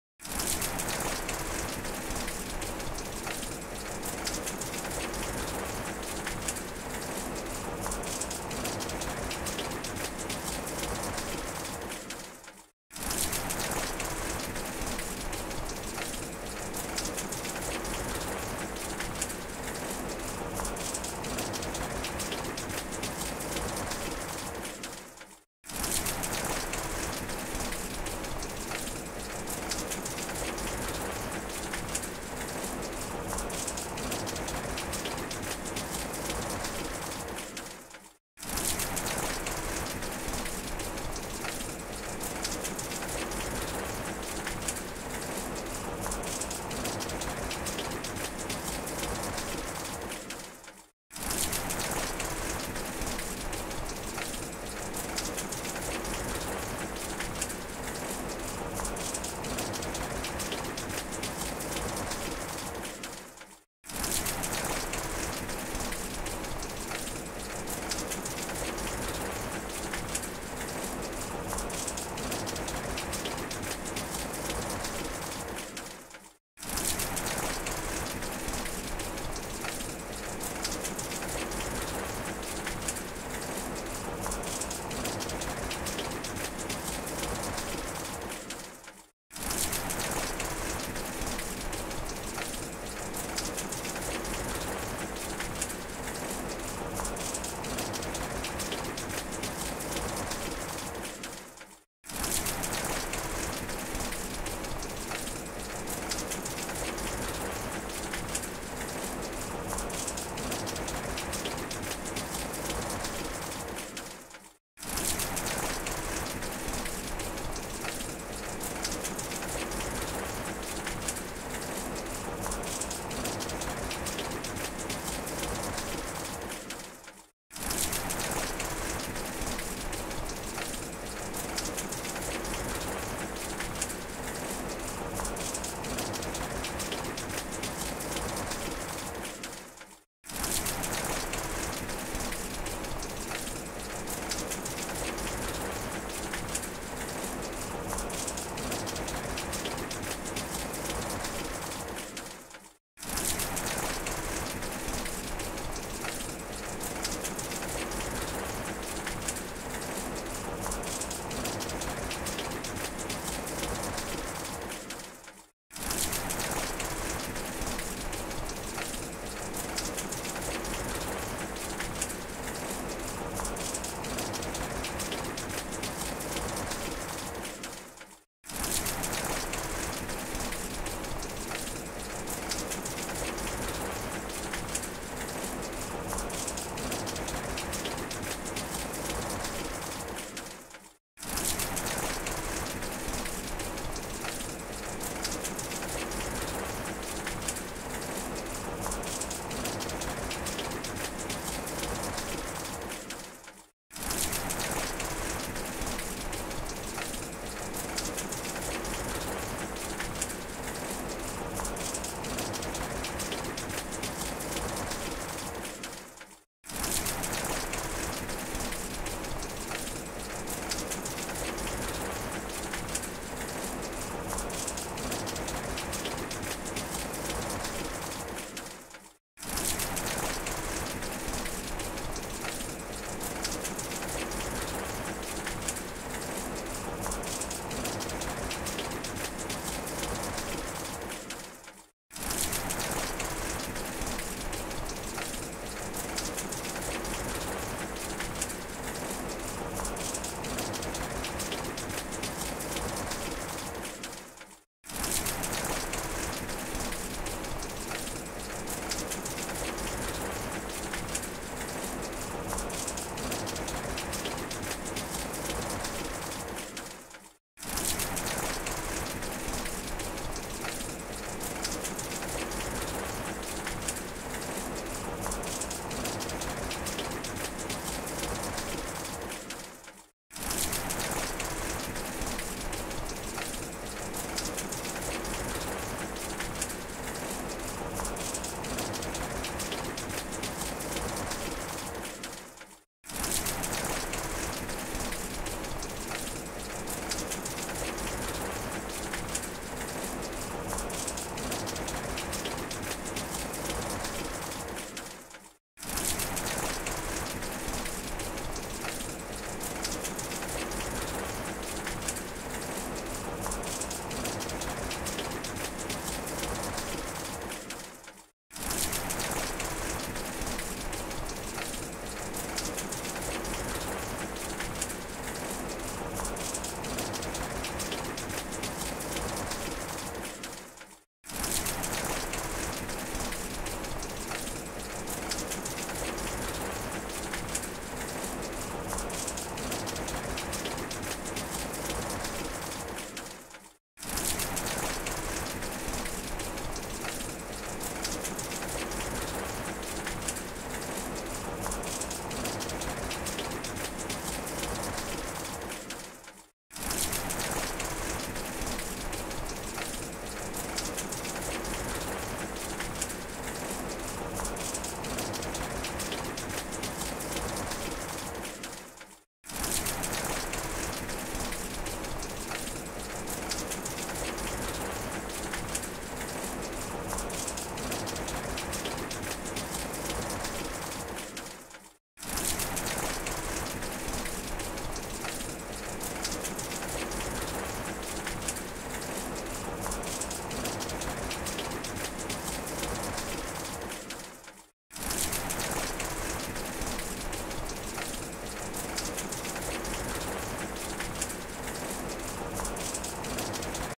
The Relaxing Calming Effect of Rain
Do you know that falling rain has a relaxing, calming effect?
Researchers and psychologists refer to falling rain as 'pink noise,' a type of sound sleep.
It acts as a natural sedative for the active mind, blocking out other sensory information such as sirens, trains, and anything else that keeps the mind from calming down.
video by Jyoti Pur from Pexels